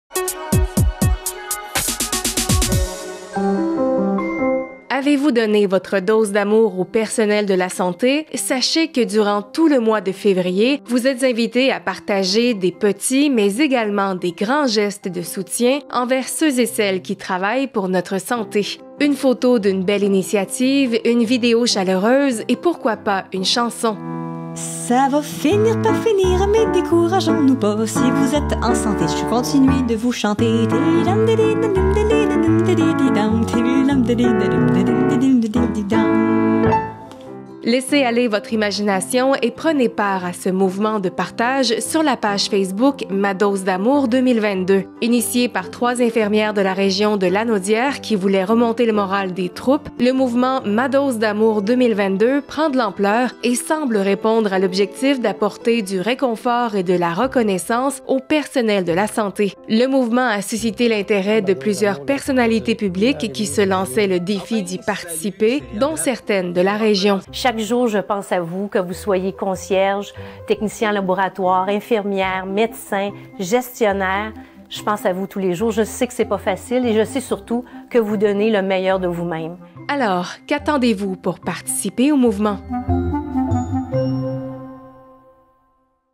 Connecté Valleyfield - Une dose d'amour pour le personnel de la santé
Avez-vous donné votre dose d’amour au personnel de la santé ? Sachez que, durant tout le mois de février, vous êtes invités à partager de petits et grands gestes de soutien envers ceux et celles qui travaillent pour notre santé.